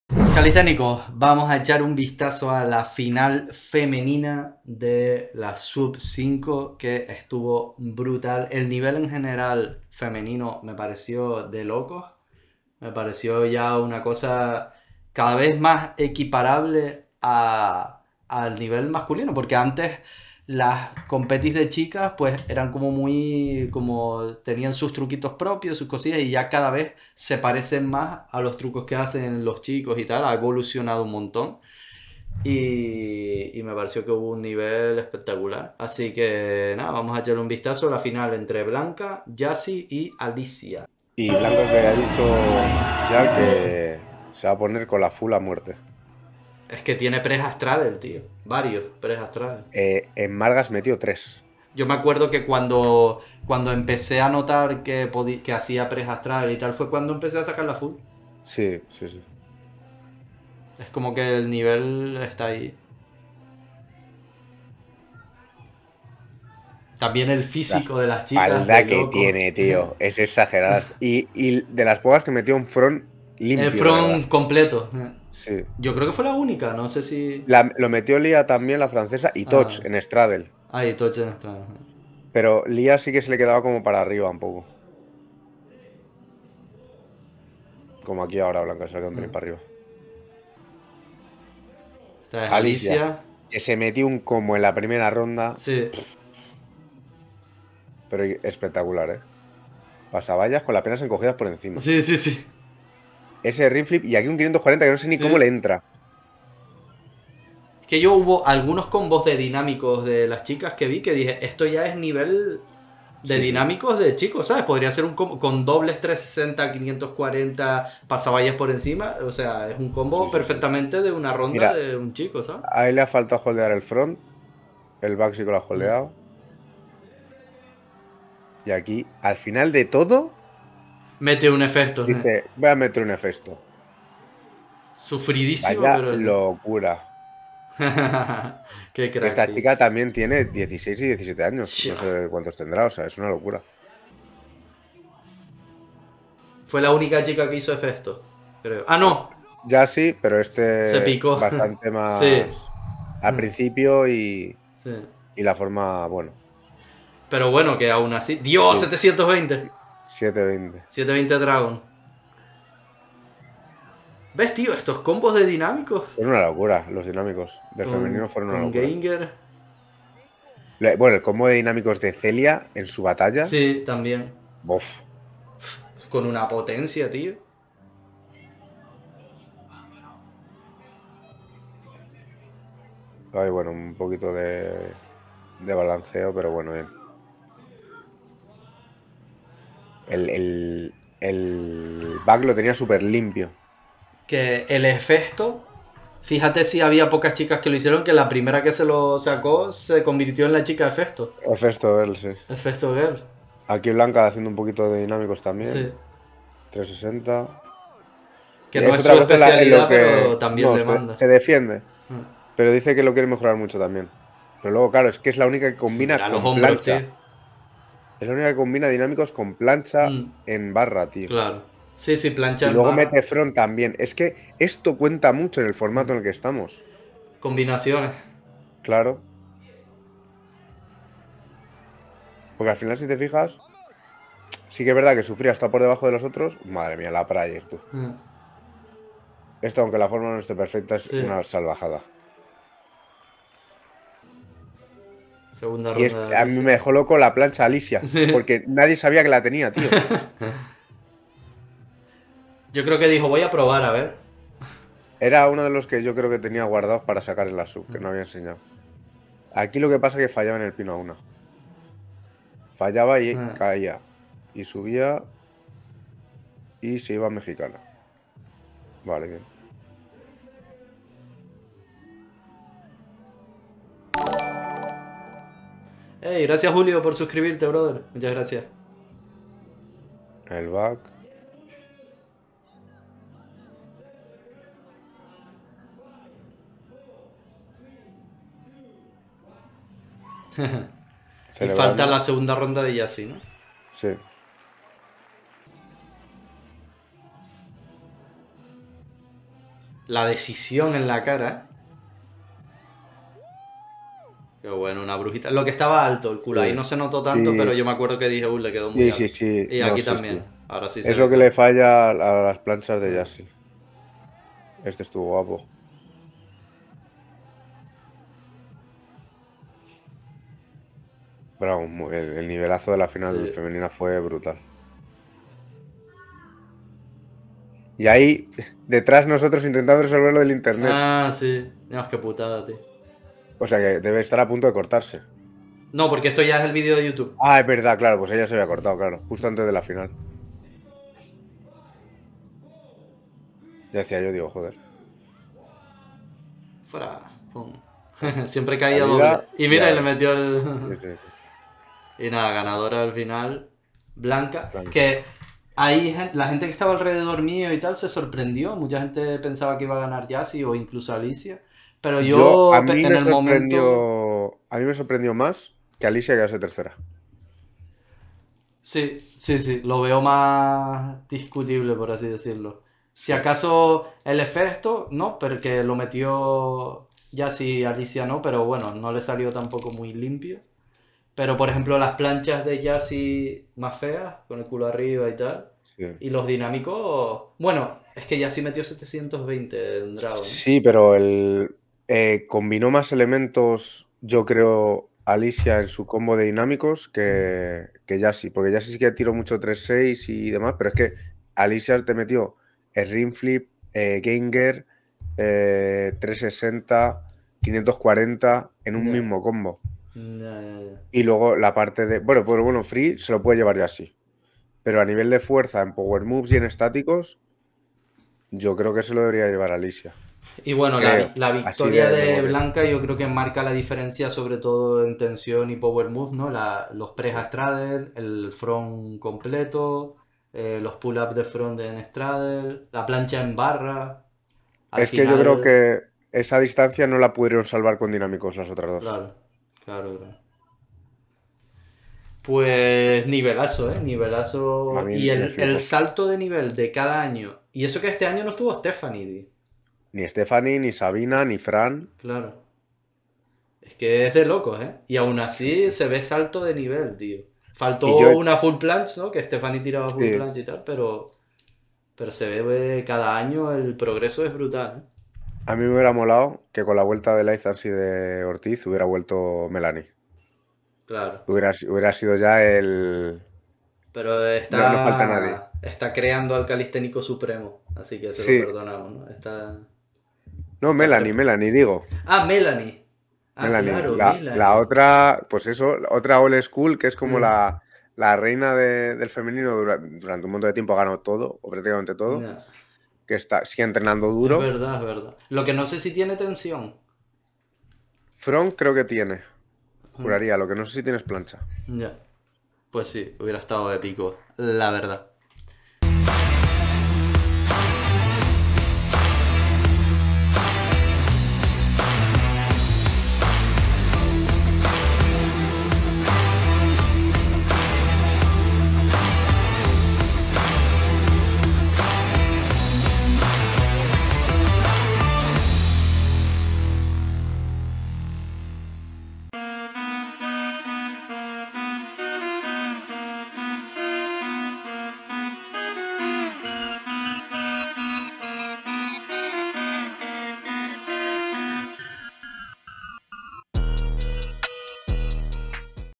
¿LA MEJOR FINAL FEMENINA DE LA HISTORIA? Jasi vs. Blanca vs. Alisia
Repasamos la final entre Jasi, Blanca y Alisia en las SWUB V internacionales, la competición más importante de Calistenia en la actualidad.

Mi libro: "La calle es tu gimnasio", con todo lo necesario para iniciarte en la calistenia. Disponible en formato digital y físico